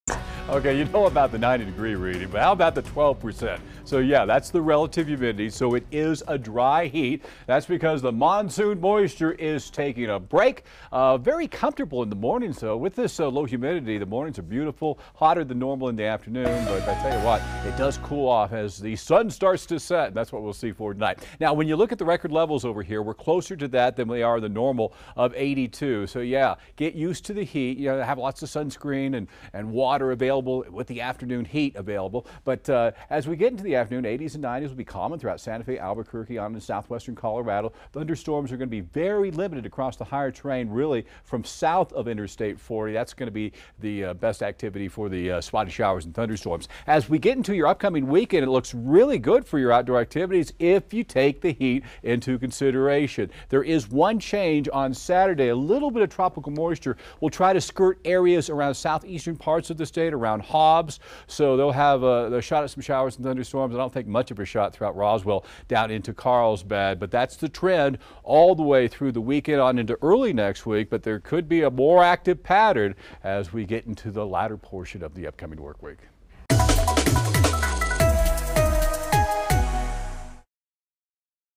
Unusually warm temperatures ahead
Chief Meteorologist Joe Diaz says we'll get a break from the monsoon moisture and have hotter than normal temps for this time of the year.